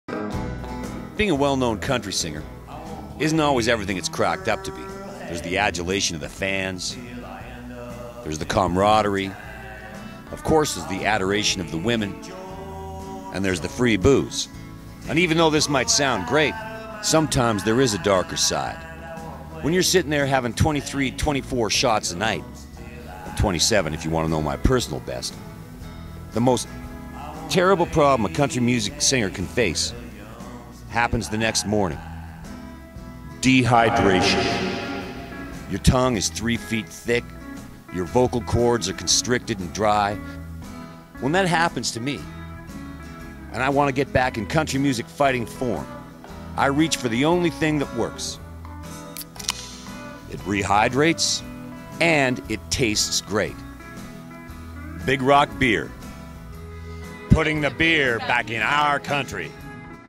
Big Rock Eddies Entry Tom Phillips & Thin Soapy Films
Big Rock beer commercial for the Annual Eddies contest (2008) featuring Tom Phillips of Tom Phillips and the Men of Constant Sorrow. Directed by Dave Reynolds, shot by Allen Woo, Edited by Regina McCreary